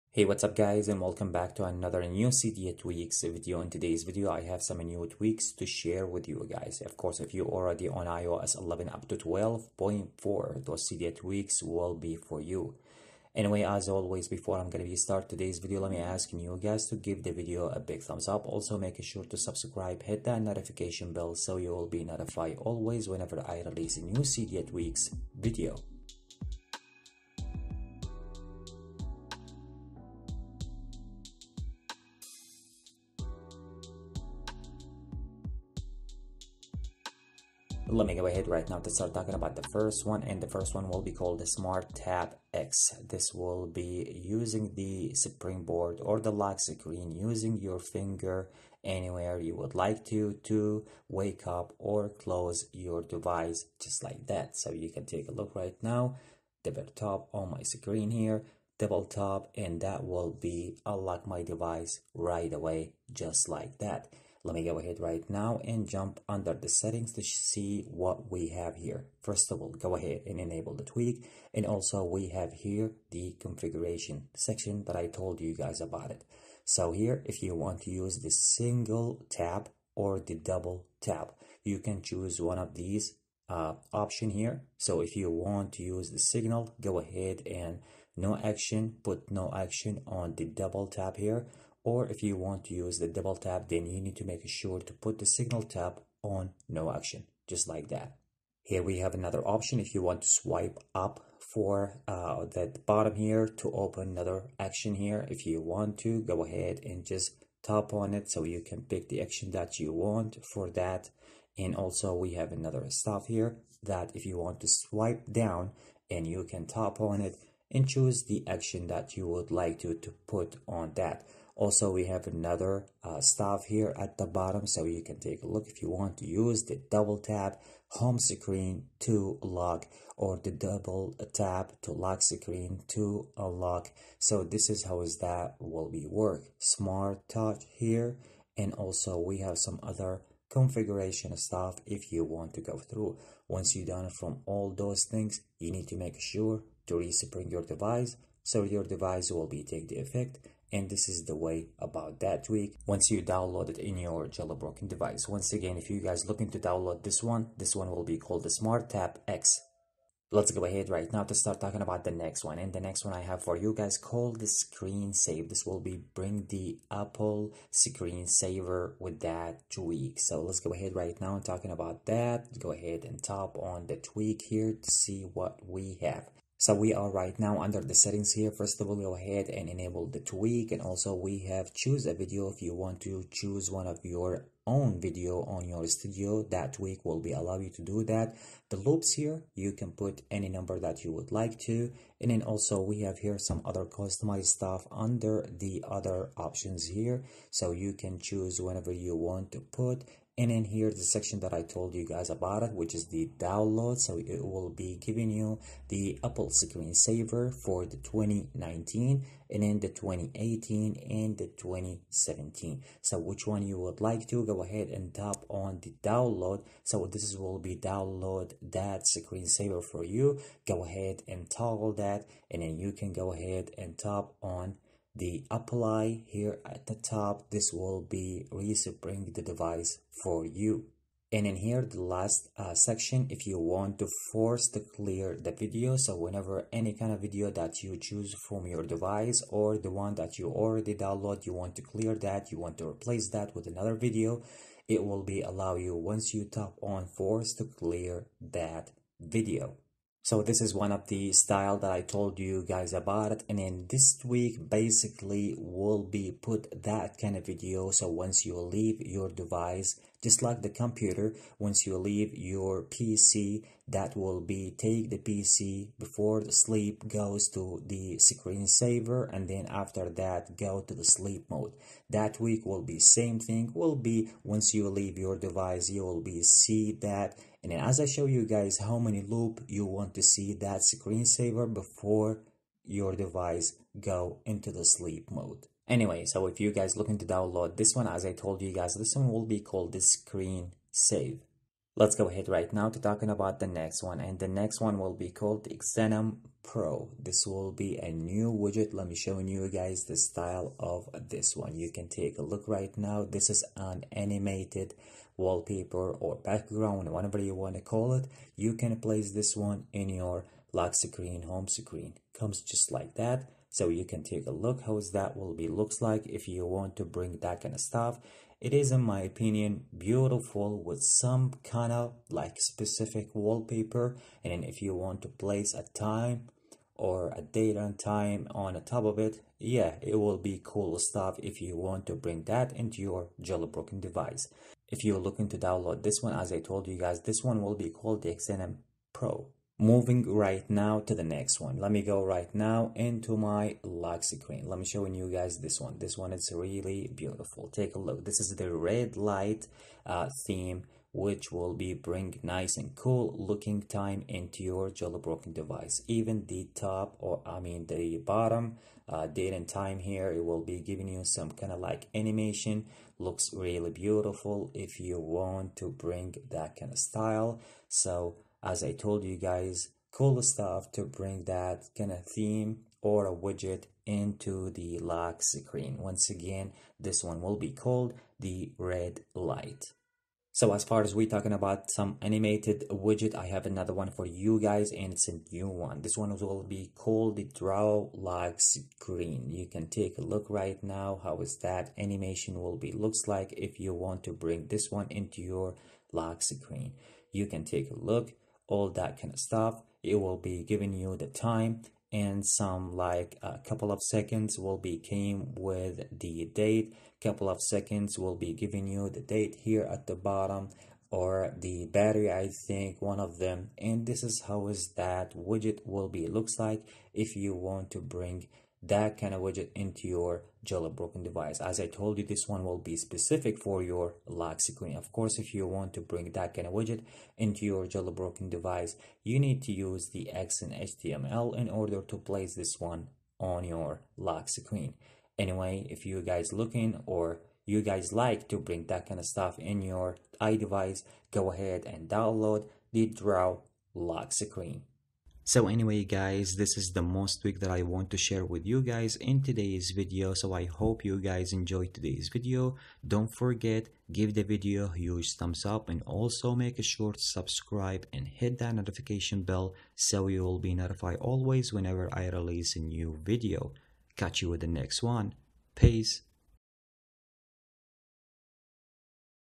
NEW Jailbreak Tweaks unc0ver and sileo iOS 12- 12.4! (Cydia & Sileo)
iOS 12.4# Perfect Jailbroken iPhone: NEW  Jailbreak Tweaks# for Cydia# & Sileo#.

My Theme: Pollux Icon